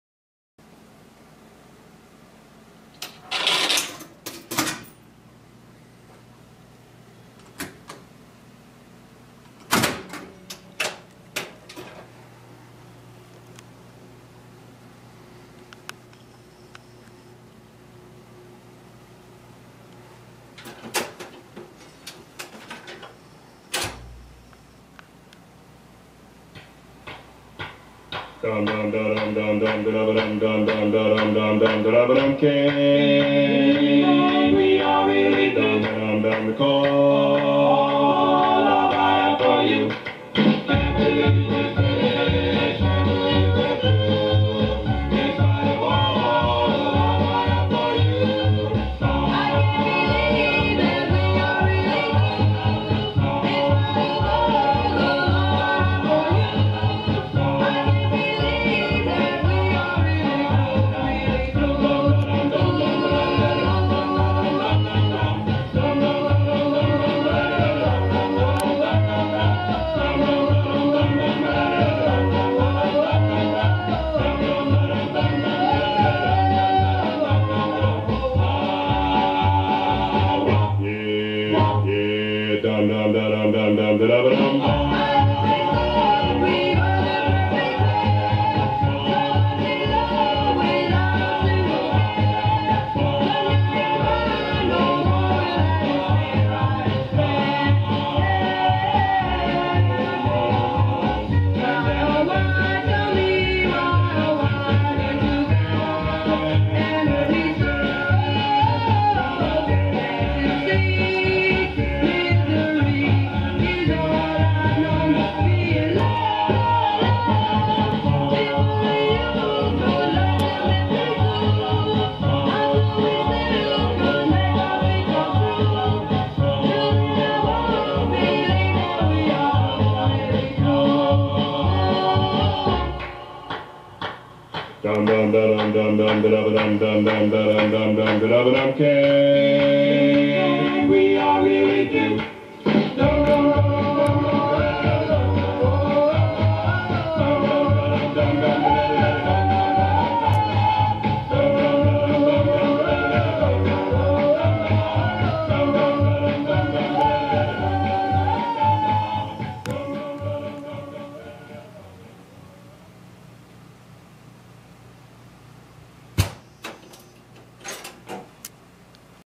seeburg W 1953 restored
1953 seeburg 100 W
My first attempt at a full restoration .
It sounds real good with a big sound.